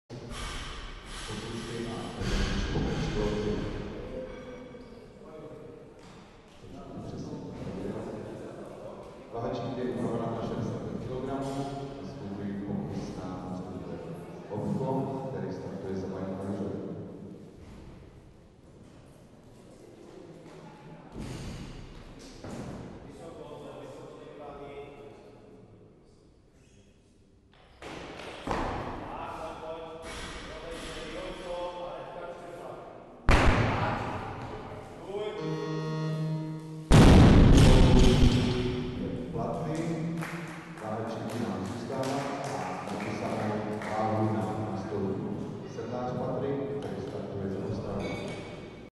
Hopko Jan (2000), NH2, 65 kg
ČEZ liga juniorů ve vzpírání družstev - 1. kolo, skupina B

Místo: Hala vzpírání, Havířov

Federace: ČSV
Pořadatel: SKV Baník Havířov